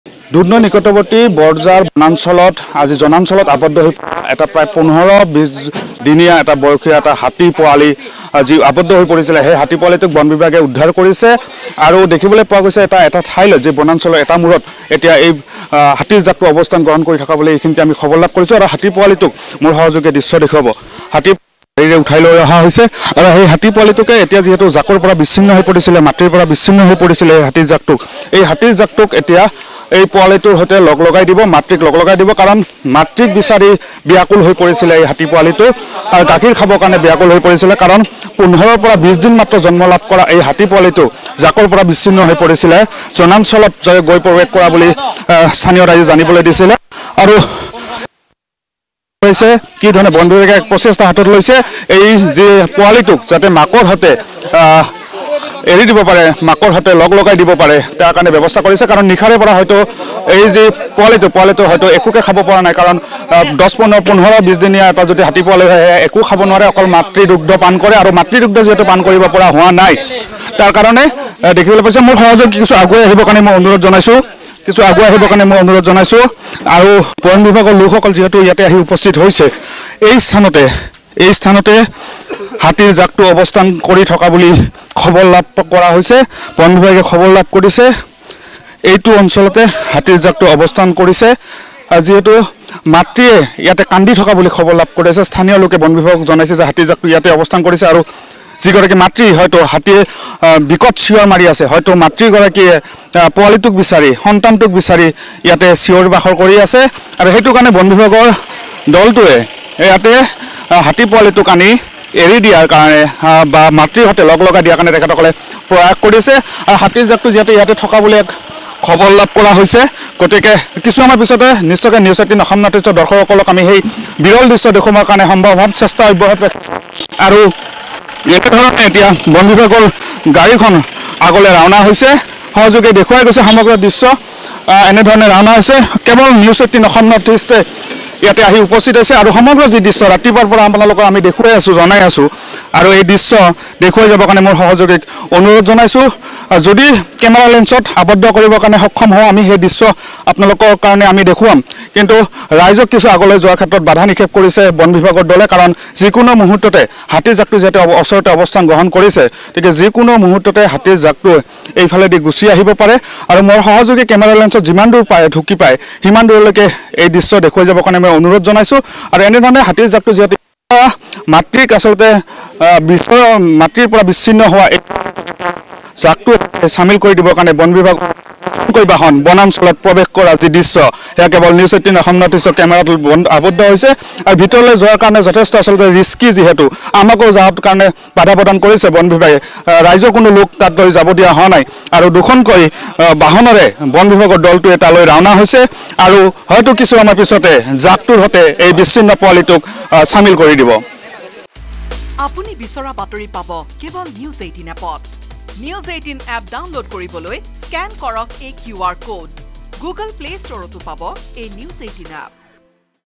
Elephant Calf Rescued : দুধনৈৰ বৰঝাৰত আৱাসিক এলেকাত আবদ্ধ এটা বনৰীয়া হাতীৰ পোৱালি | N18V
Elephant Calf Rescued | দুধনৈৰ বৰঝাৰত আৱাসিক এলেকাত আবদ্ধ এটা বনৰীয়া হাতীৰ পোৱালি ৷ ৰাইজে আবদ্ধ কৰি ৰাখিছে পোৱালিটো৷ বৰঝাৰত আছে হাতীৰ এটা বৃহৎ জাক ৷